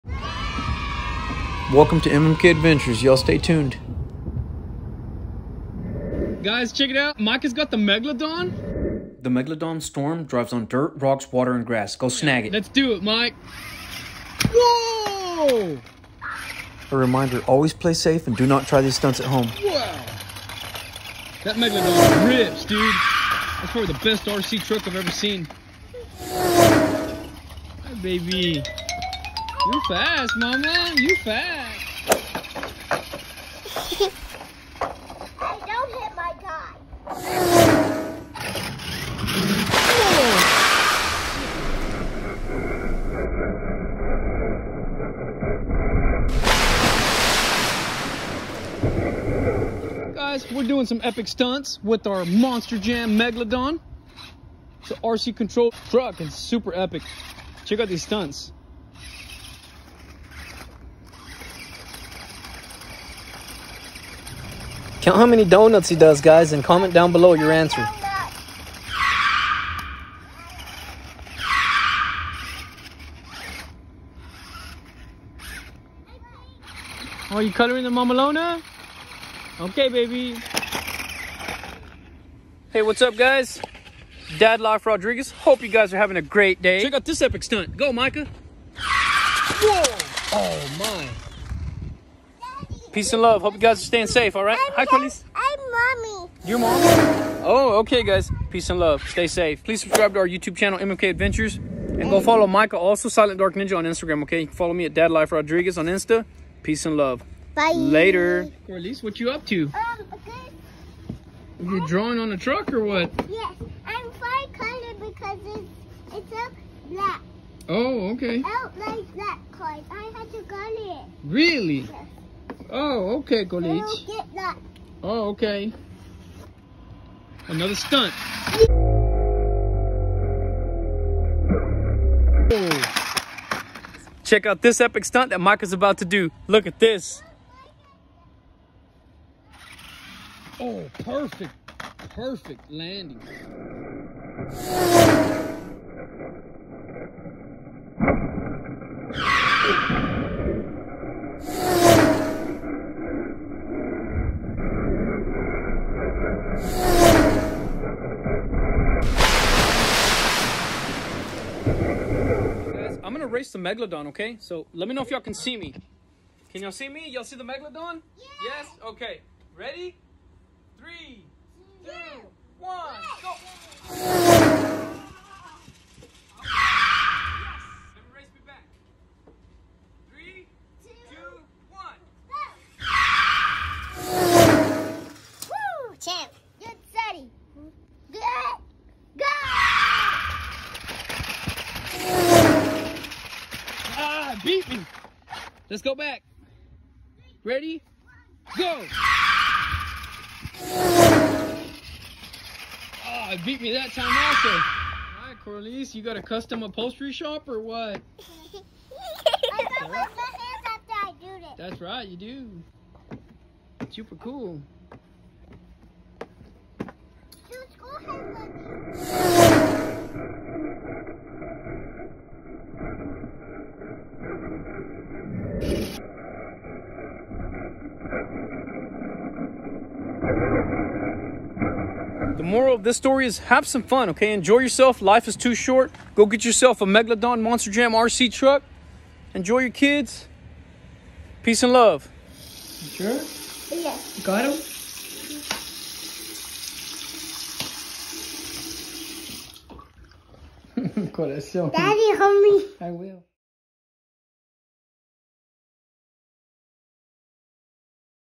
MEGALODON Monster Jam RC truck is a must have! (REVIEW)
Micah and Koralise are playing in the back yard and decided to Review the Megalodon Monster jam RC truck. It can drive on water, mud, grass and rocks! Go snag it ASAP! This is not a paid promo…